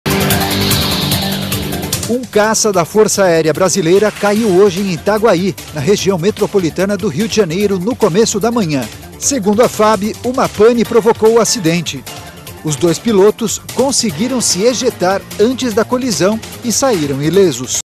Avião da Força Aérea Brasileira cai às margens da Rio-Santos
Um avião da Força Aérea Brasileira, a FAB, caiu hoje de manhã na região metropolitana do Rio de Janeiro.